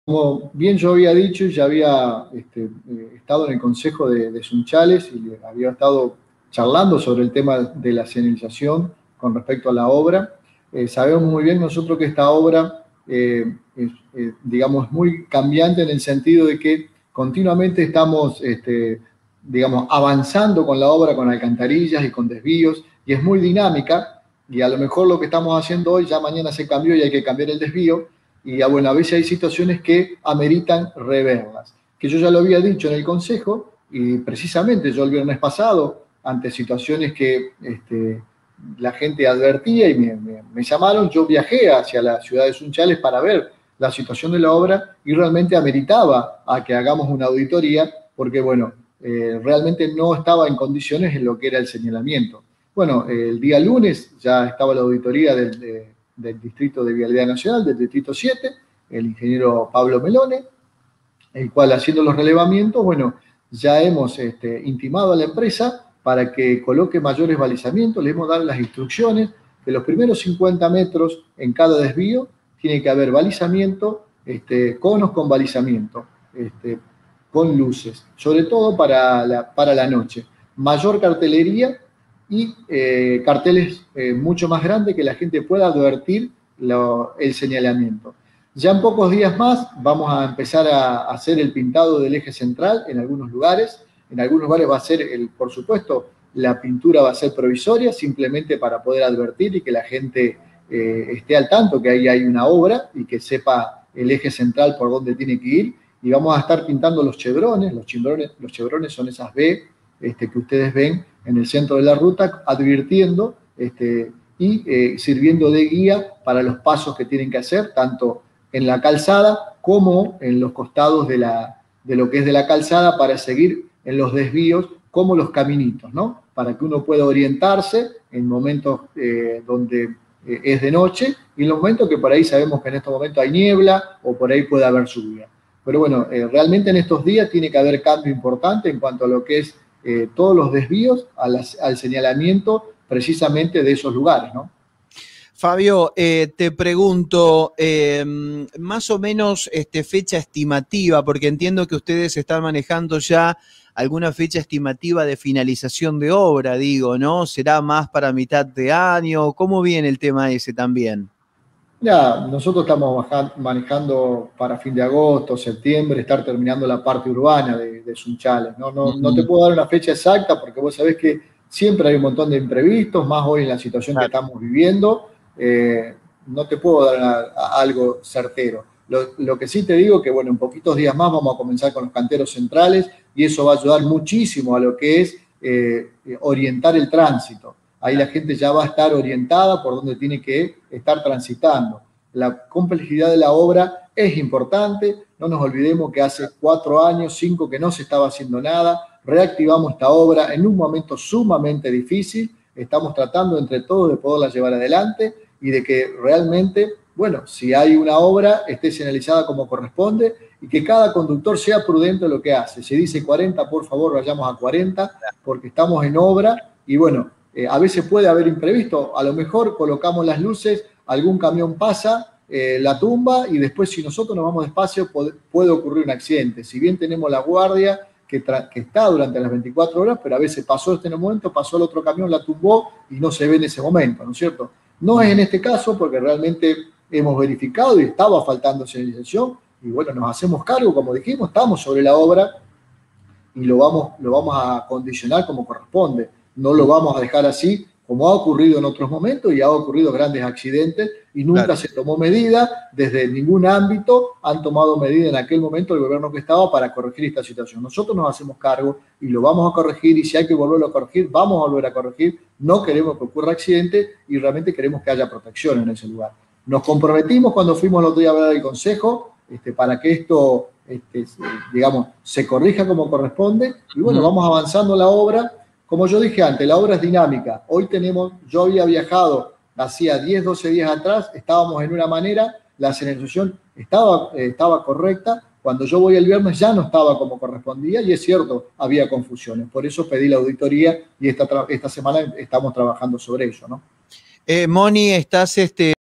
Fabio Sánchez jefe zona 7 Vialidad Nacional
DICTAMEN DE LA AUDITORÍA  sobre la señalización del tramo urbano a la ciudad. ZONA DE OBRAS.